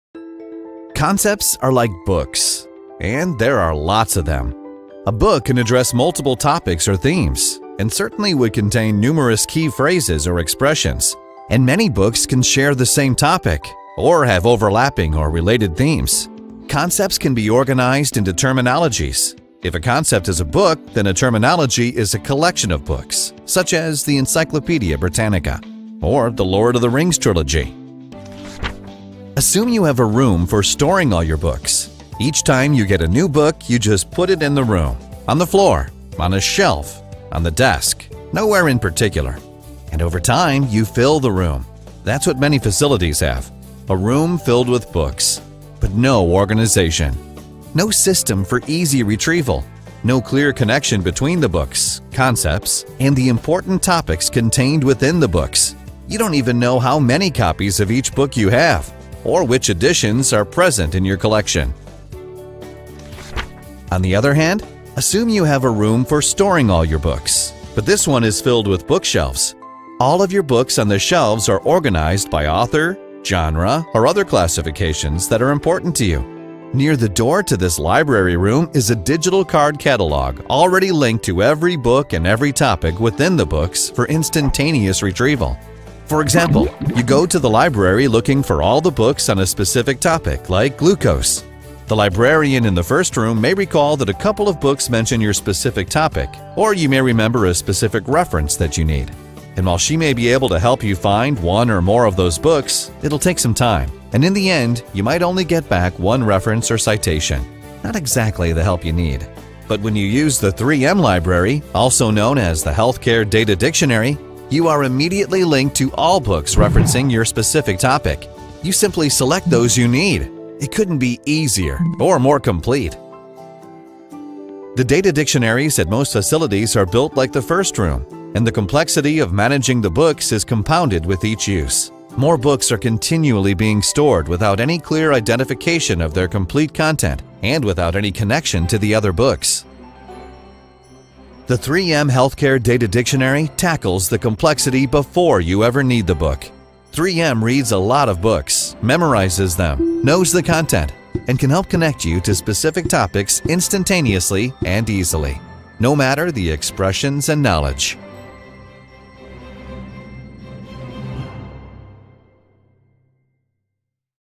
Mapping done right
Bring order to chaos with 3M's Healthcare Data Dictionary. Integrate your data regardless of how it is collected or stored in legacy systems.